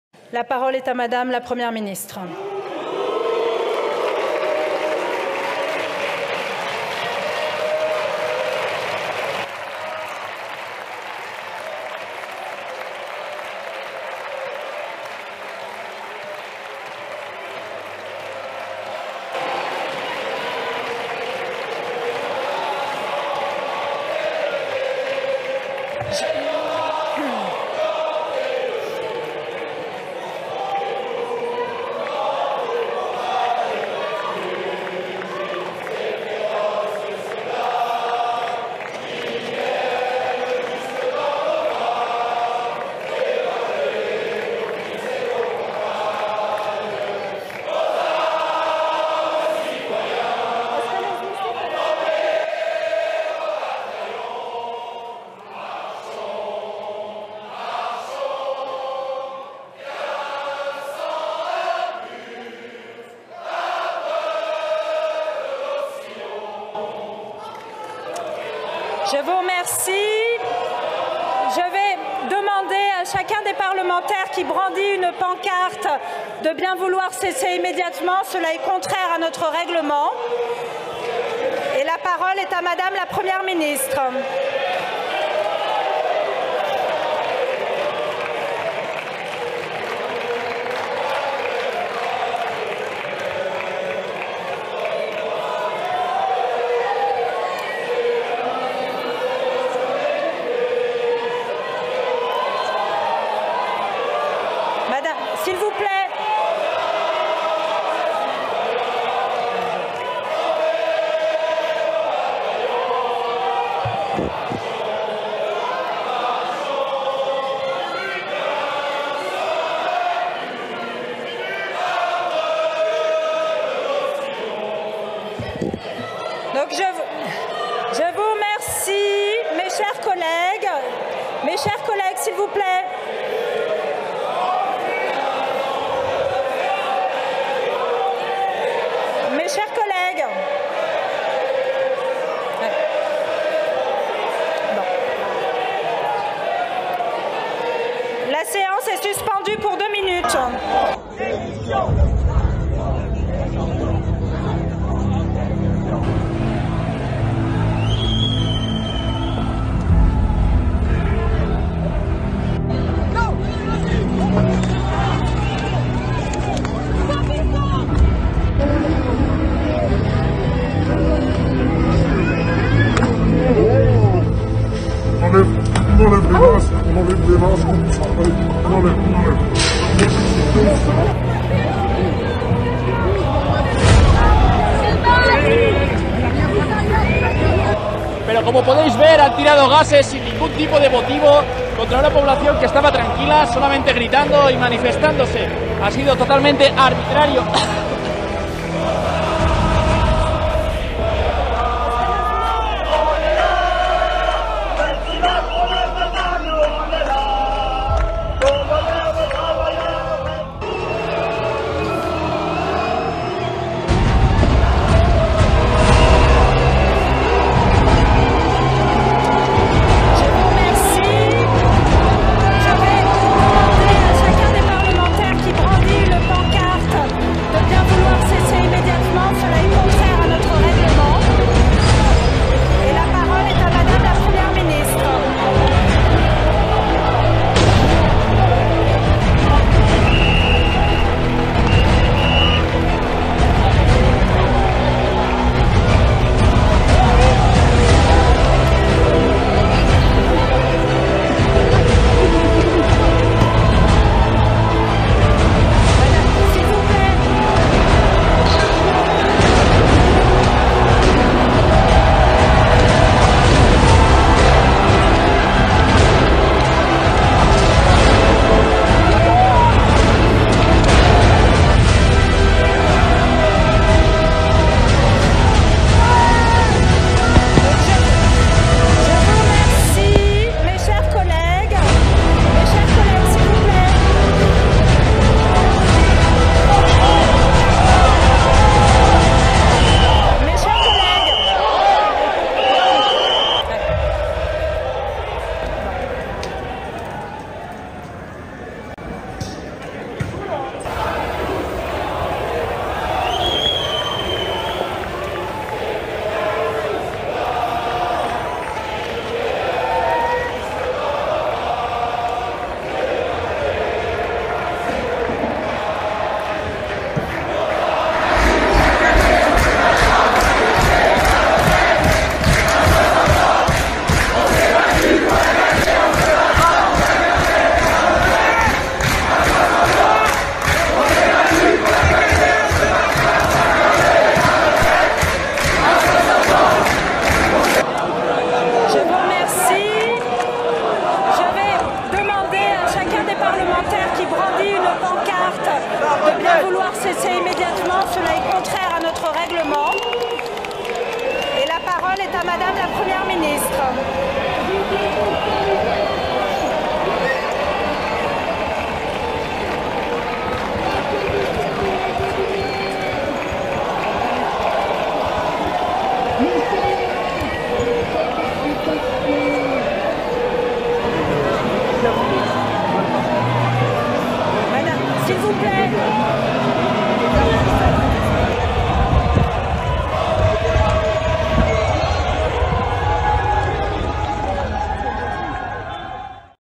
Arde Francia por el incremento de la edad de jubilación sin autorización parlamentaria
Francia
Francia Insumisa
París
Jubilación
Arde París
Arde Francia